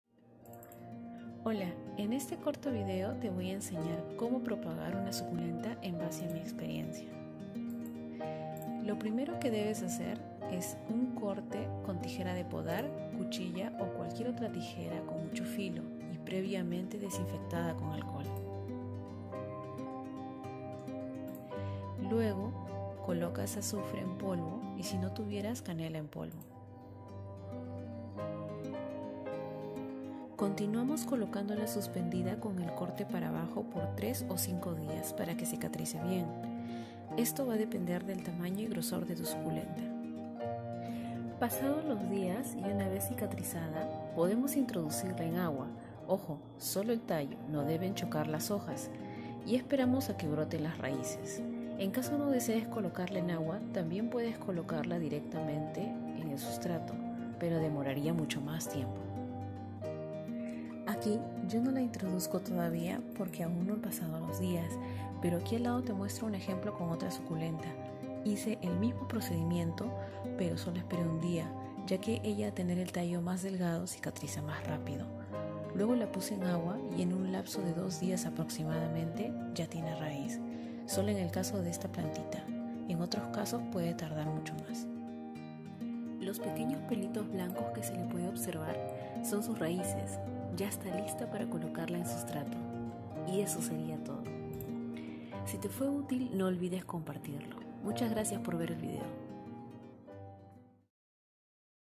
Cómo propagar suculentas por corte de tallo 😊
Aquí te muestro cómo reproducir una suculenta por corte de tallo. También aplica para cuando vamos a salvar a una suculenta por pudrición y cortamos el tallo.  😉
Paso 1: Cortar el tallo
Paso 2: Ponerle azufre o canela en polvo
Paso 3: Esperar a que cicatrice
Paso 4: Colocar sólo el tallo en agua
Paso 5: Esperar a que salgan raíces
Paso 6: Ya puedes ponerla en sustrato

Pd. Me disculparán si algo no está claro, o el volumen de mi voz es muy bajo. Es mi primer video y son las 4am, decidí hacerlo a esa hora, ya que el resto del día hay mucha bulla  😁